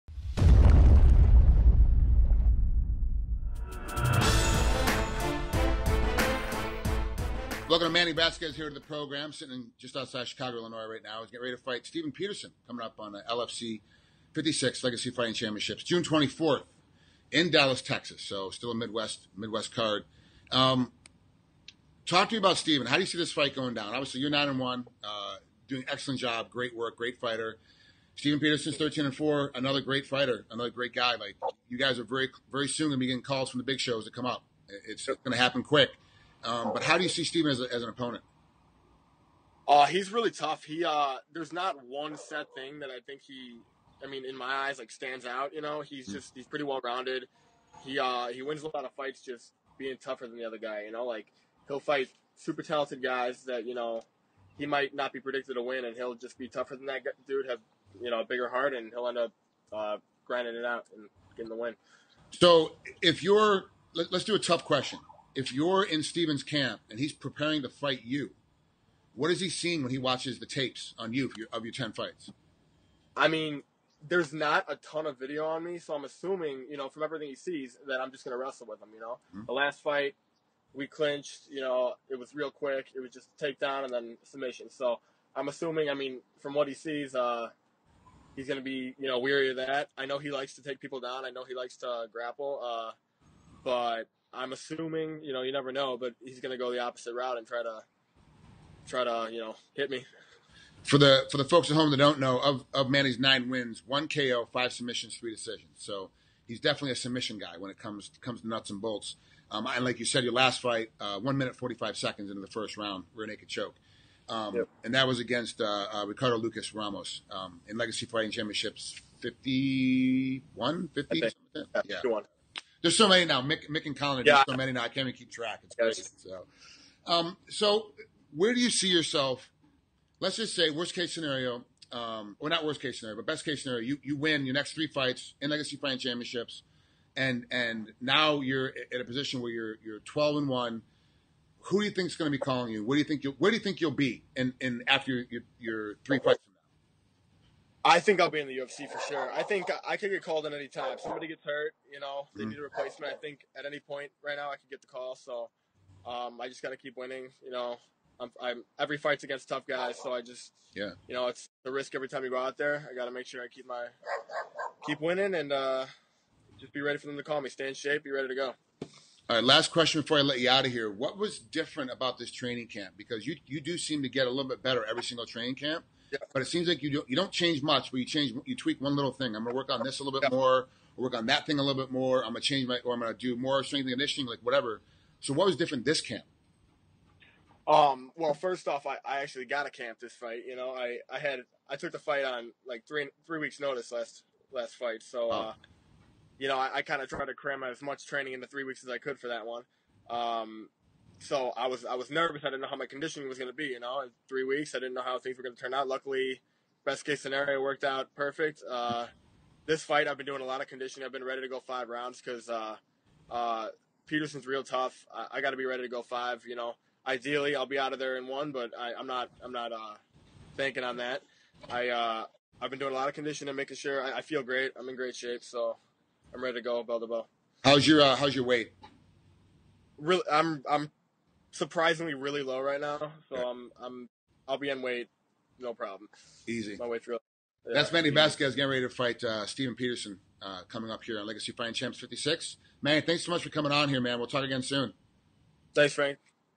Legacy FC 56's Manny Vasquez: 'I could be called up to the UFC at any time'
MMAOddsBreaker's Frank Trigg speaks with Legacy FC 56 bantamweight Manny Vasquez as he prepares for his upcoming main event bout this Friday night (June 24, 2016) against Steven Peterson at the Bomb Factory in Dallas, Texas.